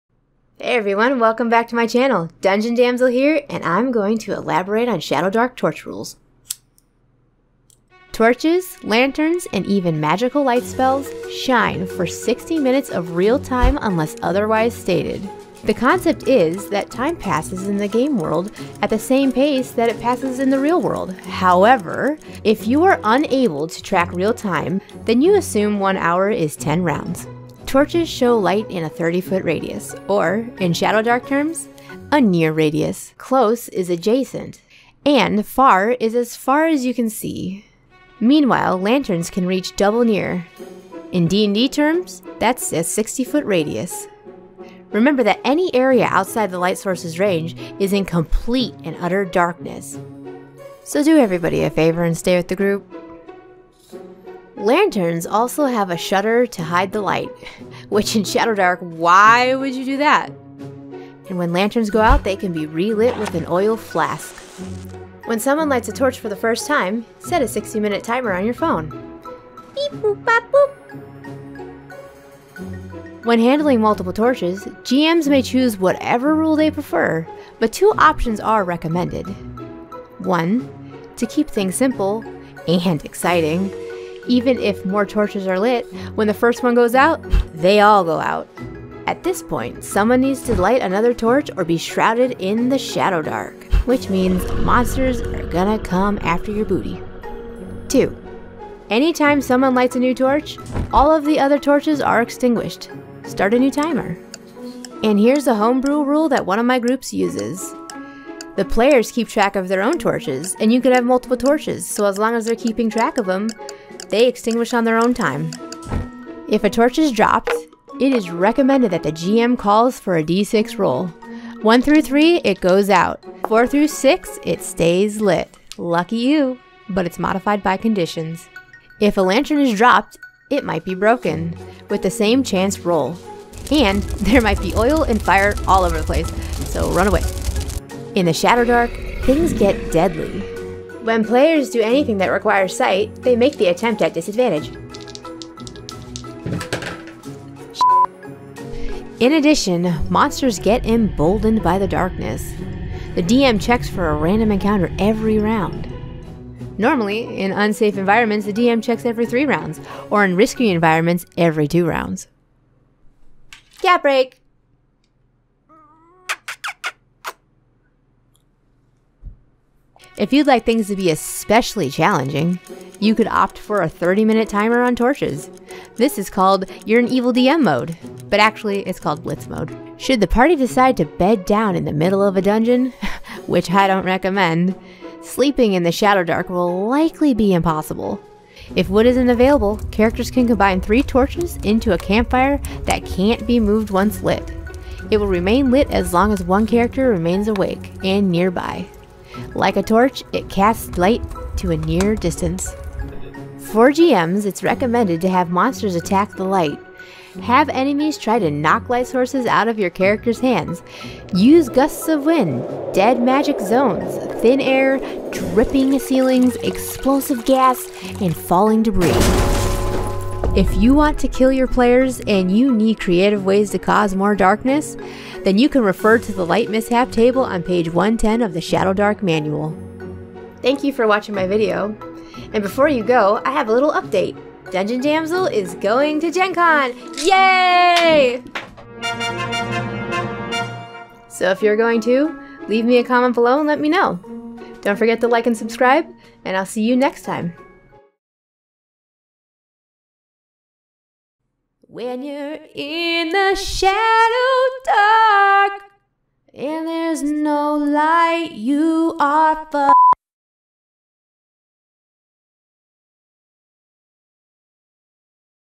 Shadowdark Torch Rules Explained
In this informative video, Dungeon Damsel outlines all of the torch, lantern and lighting rules that make Shadowdark unique! These rules include the shadowdark timing rules, Shadowdark campfire rules, Shadowdark multiple torch rules, using timers to keep track of real time torches and magical light, and instruction for GMs with wandering monsters and how they are affected by the light. While this is mostly for Shadowdark newcomers and Shadowdark beginners, even Shadowdark veterans may learn something they overlooked.

0:00 Intro
0:08 Torch Time
0:28 Torch & Lantern Radius
0:55 Lantern Rules
1:13 Multiple Torches
1:56 Torch Mishaps
2:18 In the darkness
2:43 Cat
2:50 More challenges
3:10 Campfires
3:24 For GMs
3:51 Closing
4:11 Secret Singing